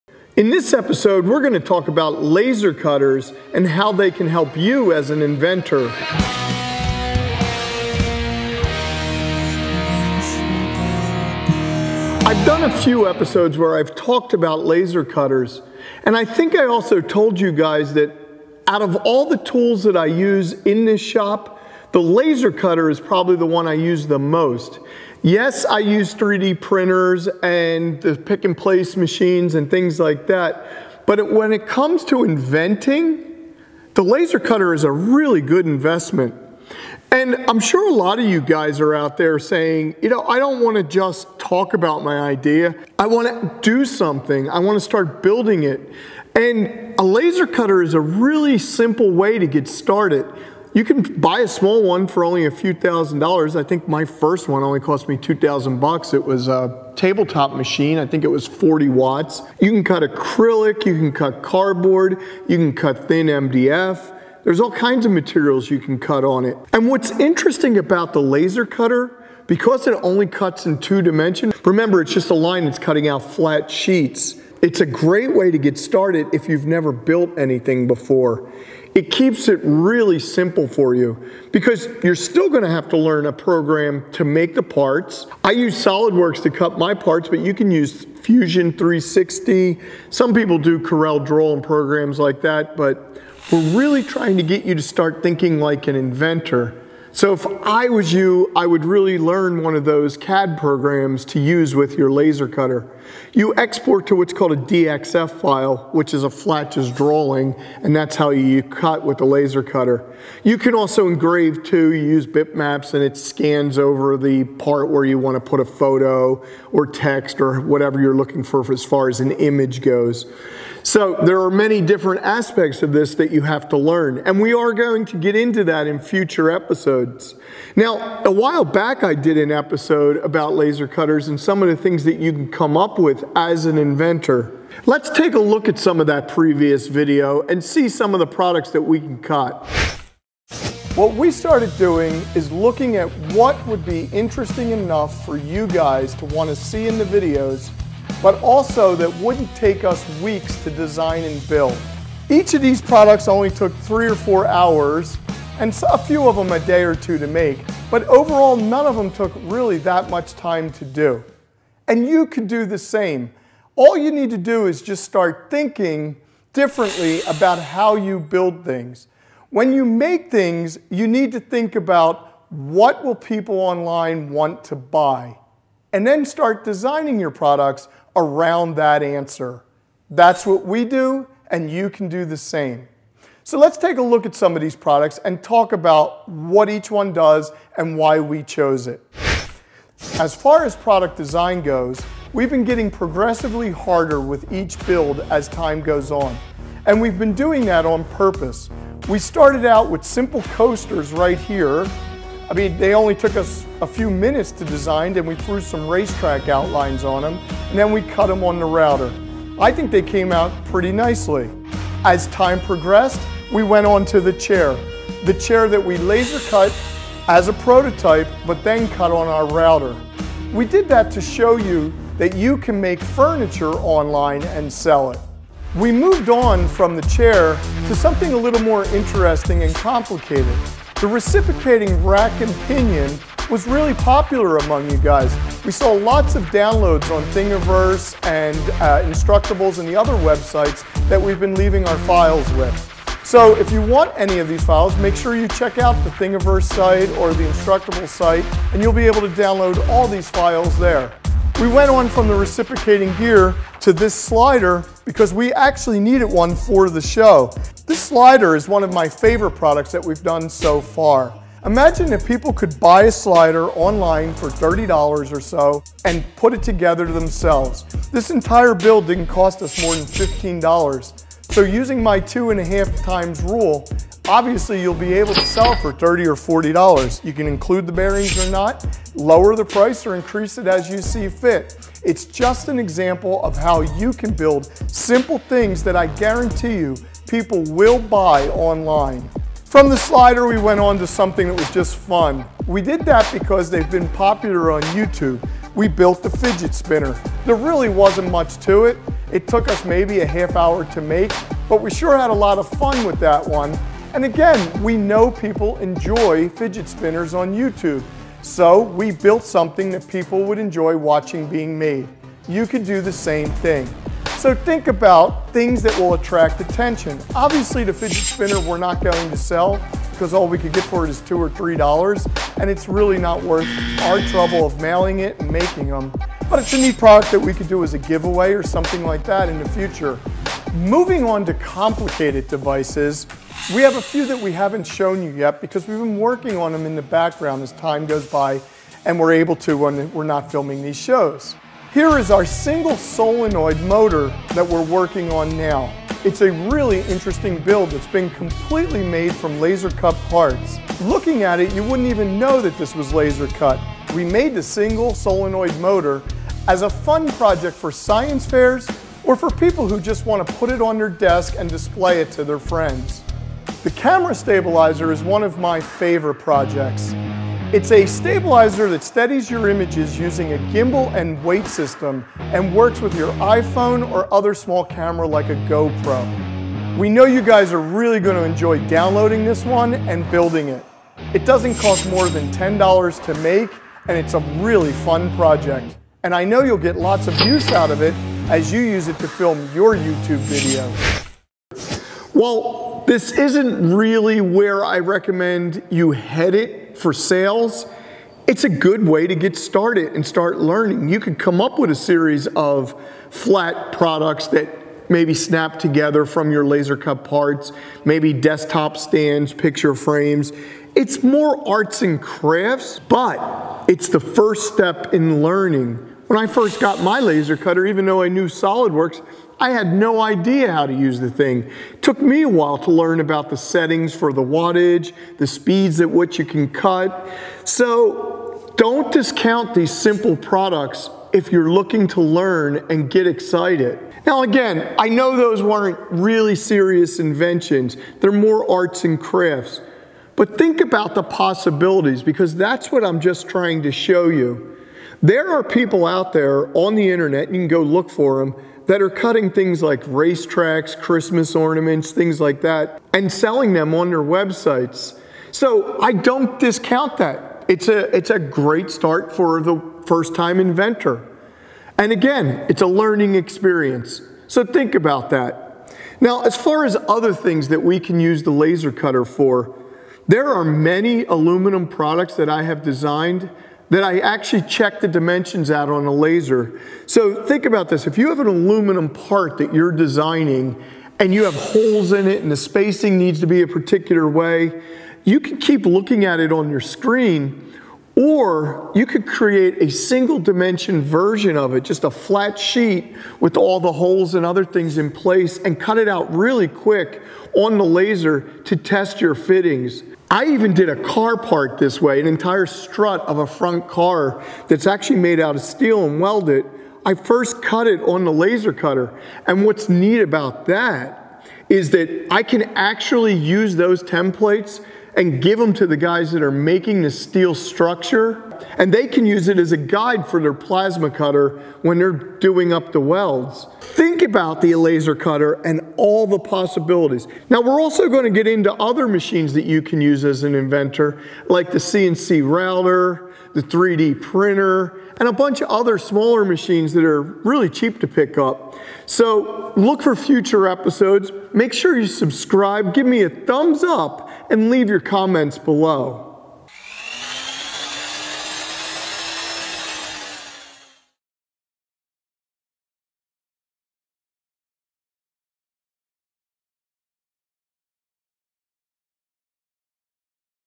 Laser Cutter And Laser Engraver Invention Ideas For DIY Projects In Your Workshop (Part 2)
This is a follow up video about how we use a laser cutter in our invention designs.  We hope that this video will give you some great ideas.

Invention Therapy is the show where we turn your ideas into a reality!